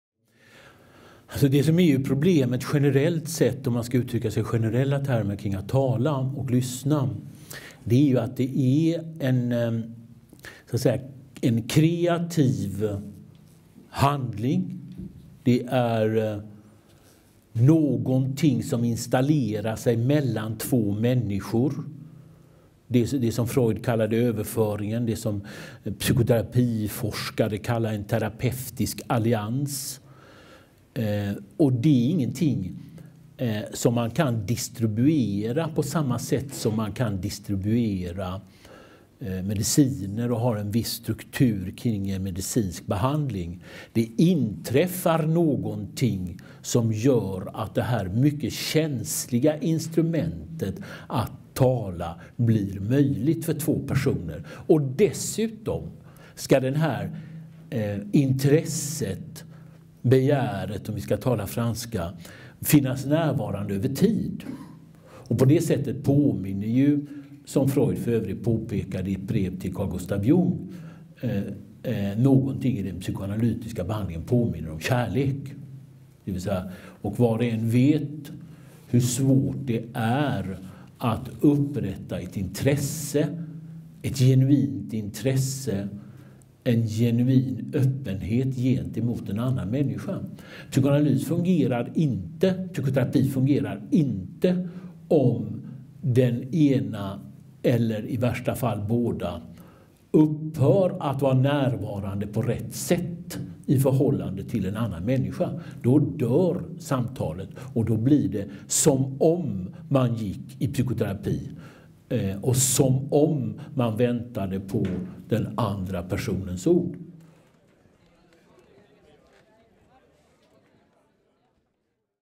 Per Magnus Johansson om vad som krävs i ett psykoanalytiskt samtal.
Per Magnus Johansson svarar på frågan vad som krävs i ett psykoanalytiskt samtal. Från ett samtal med Clarence Crafoord, Unga Klaras scen, mars 2015.